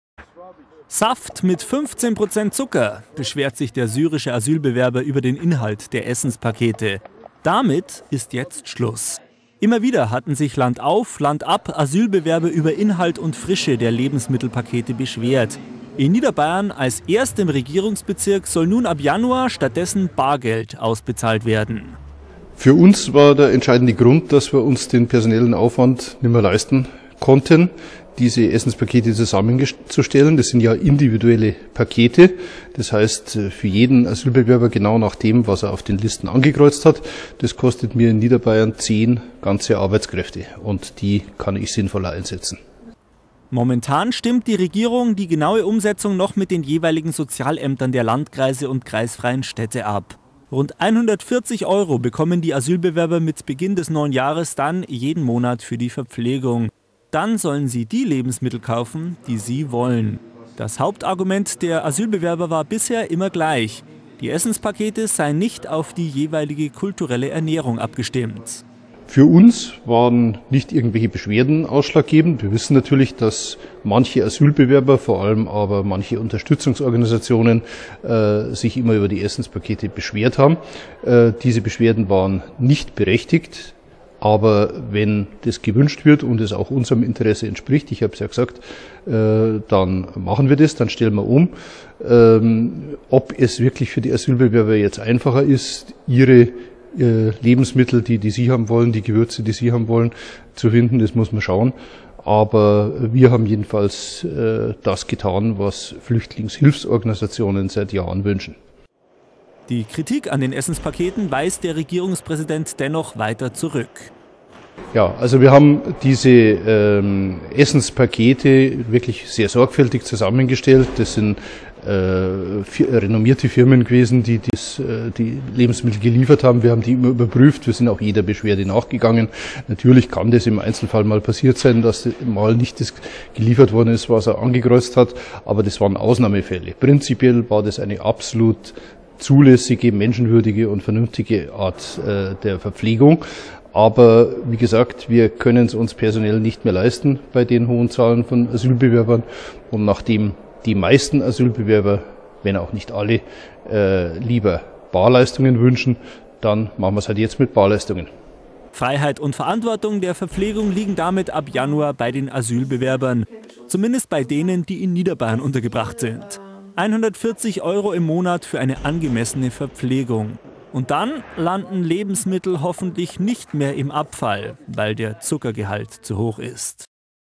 Vor der Umstellung - Geld statt Essenspakete für niederbayerische Asylbewerber
Essen und Trinken halten Leib und Leben zusammen. Für viele Asylbewerber, die meist lange Monate in Gemeinschaftsunterkünften
ausharren müssen, mag diese leichtfertig dahingesagte Lebensweisheit wie Hohn klingen. Denn ihr Essen kommt jeweils für mehrere Tage in der Plastiktüte oder im Pappkarton. In wenigen Wochen ist Schluss damit. In Niederbayern gibt es - als ersten Bezirk in Bayern - ab Januar keine Essenspakete mehr. Die Asylbewerber erhalten Geld und können selbst einkaufen.